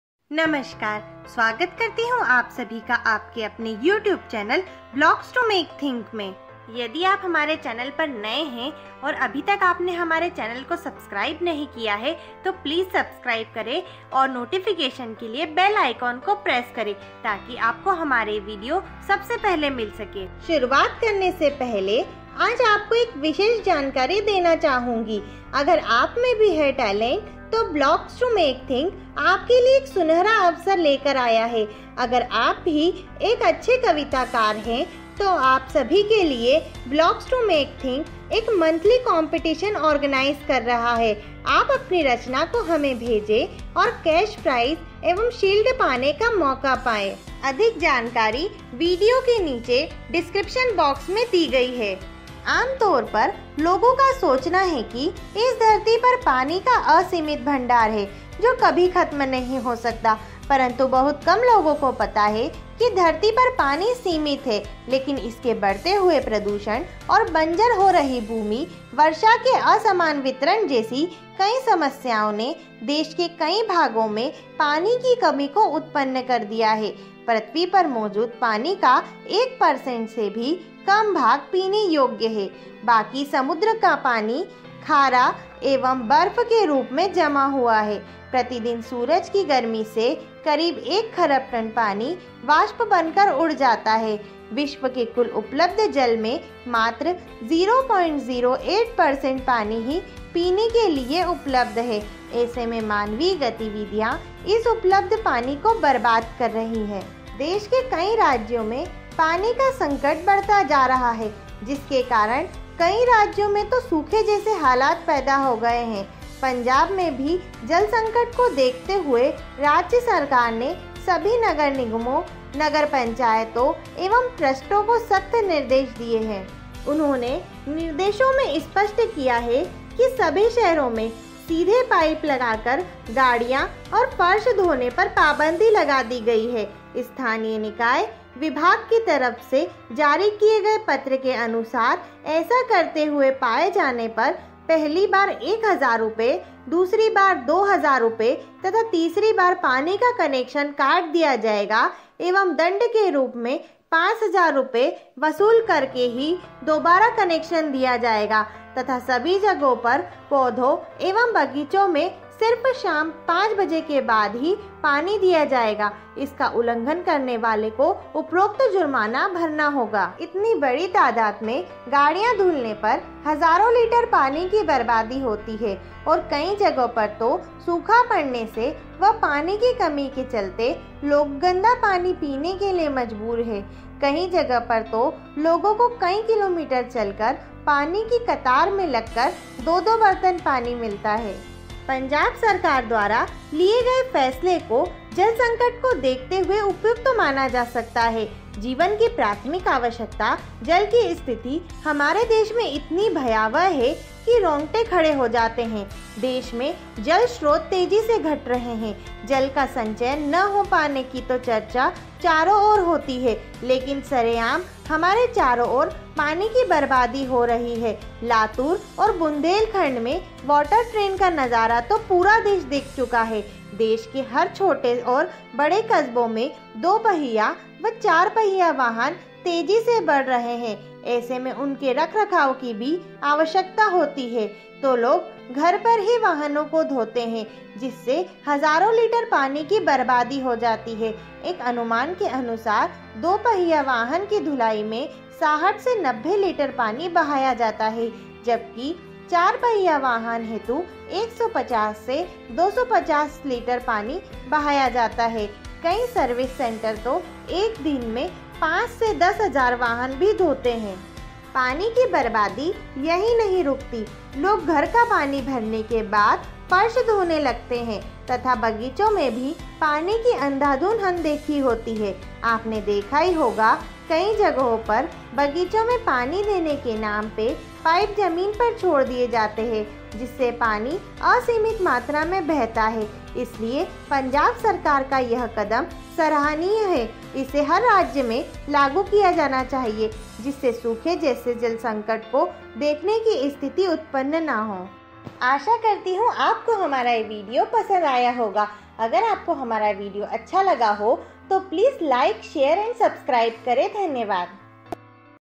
एक कदम पानी बचाने की तरफ | Steps of Chandigarh Government to SaveWater | Rishika Joshi
धरती पर पानी की कमी की समस्या दिनों दिन बढ़ती जा रही है और कुछ लोग इसे नजरअंदाज करते हुए पानी के बर्बादी करते रहते हैं । इसे रोकने हेतु चंडीगढ़ सरकार ने एक सख्त कदम उठाया है ।

जैविक इंधन | क्यो मनाया जाता है विश्व जैविक इंधन दिवस | World BioFuel Day Special | Rishika Joshi